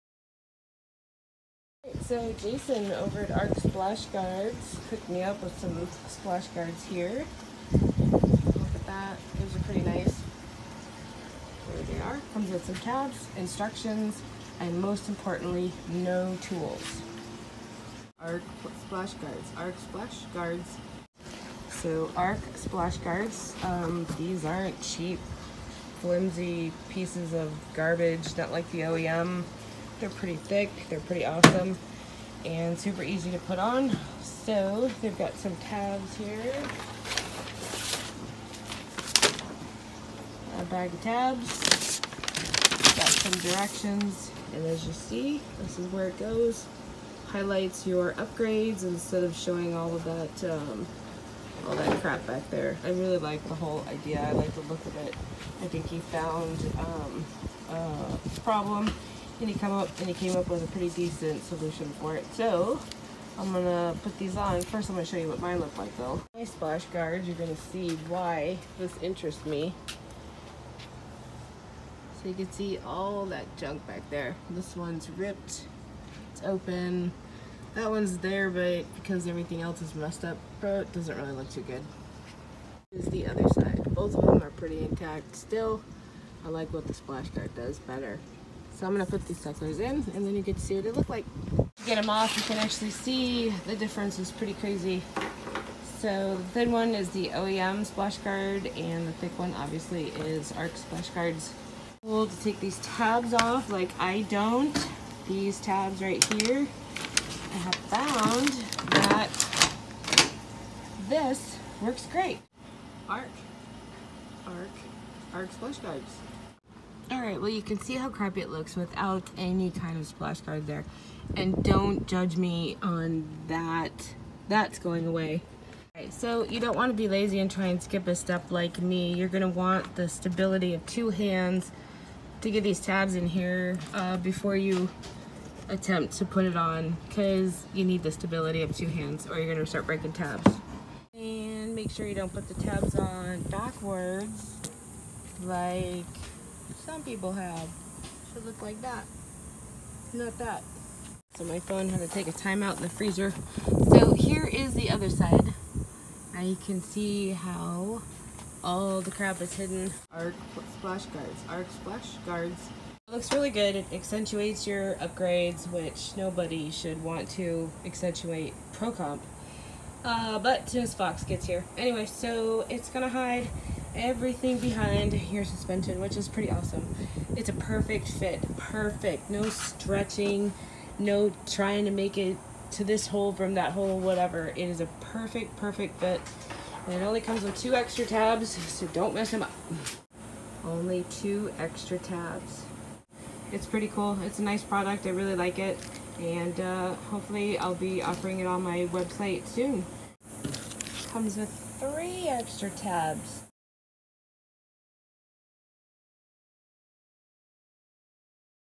Installing my new
Ark has launched these new splash guards for the Tacoma and I love them. Durable and a PERFECT FIT.

NOW AVAILABLE Ruggedfemale.com